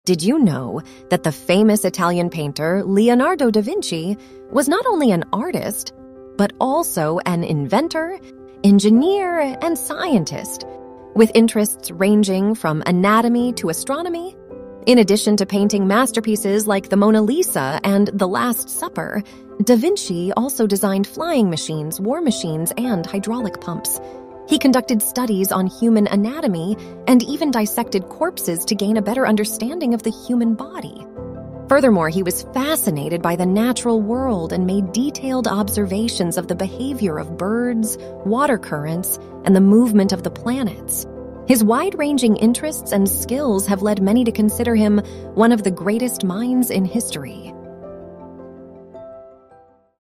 Did you know? هل تعلم؟
Did you know that the famous Italian painter Leonardo da Vinci was not only an artist, but also an inventor, engineer, and scientist, with interests ranging from anatomy to astronomy? In addition to painting masterpieces like the Mona Lisa and The Last Supper, da Vinci also designed flying machines, war machines, and hydraulic pumps. He conducted studies on human anatomy and even dissected corpses to gain a better understanding of the human body. Furthermore, he was fascinated by the natural world and made detailed observations of the behavior of birds, water currents, and the movement of the planets. His wide-ranging interests and skills have led many to consider him one of the greatest minds in history.